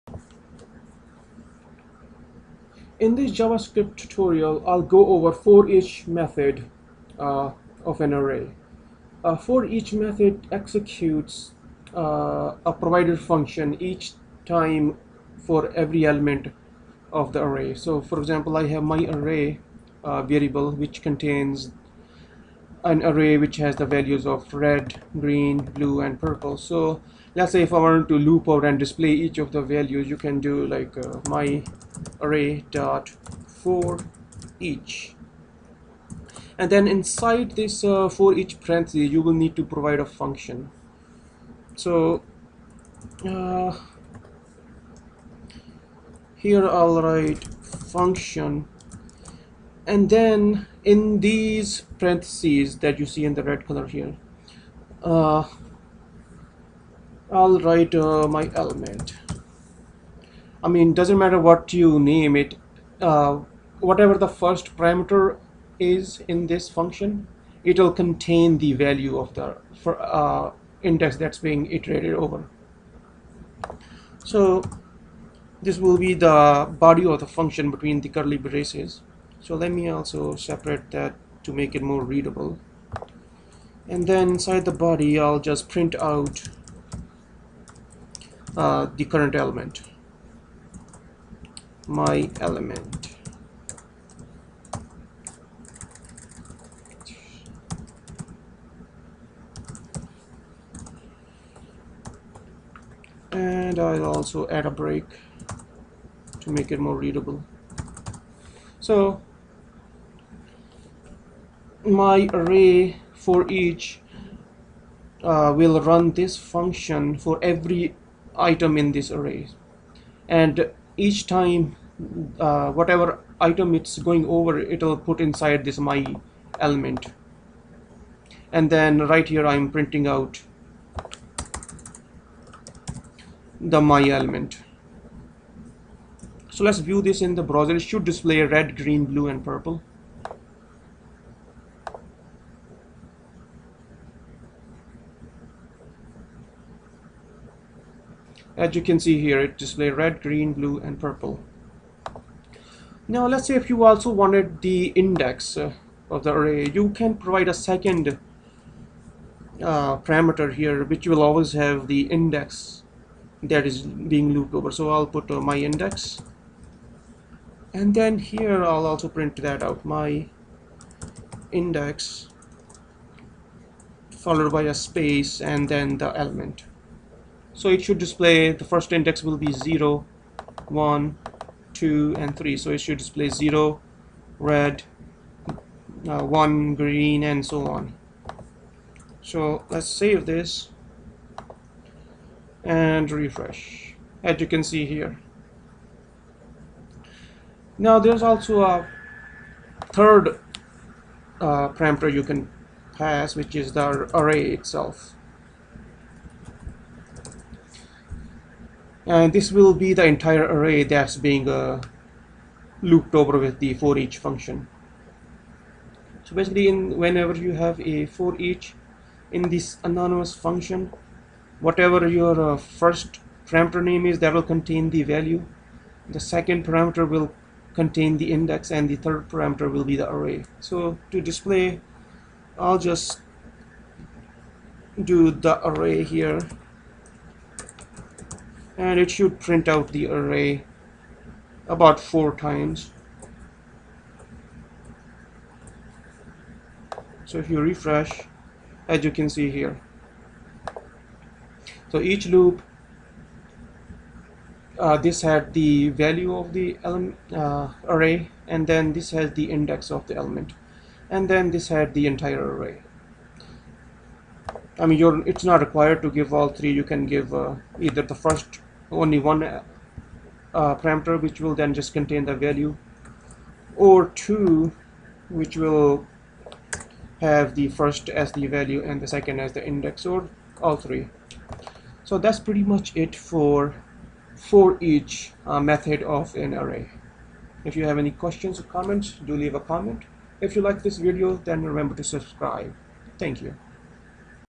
JavaScript Tutorial 15 - forEach Loop in Arrays
In this tutorial I go over forEach function of JavaScript. You can you the forEach method of an array to apply a function. For example, if you have the following array:-

myArray = [1,2,3];

then you use foreach to iterate over each value and execute any function you provide to foreach:-

myArray.forEach(you function here);